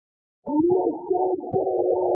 mr beast scream but it's low quality and sounds like you're under ice
you are mr beast, you are trapped in a hollow block of mostly thick but slightly thin ice after a 10,000,000 dollar challenge went horribly wrong. you have no way to get out. you've been trying for hours to escape, your hands are basically useless now because of how cold they are. you think of chandler and the others while passing out from a mixture of both hypothermia and a lack of oxygen. when suddenly, you hear from the outside of your icy prison: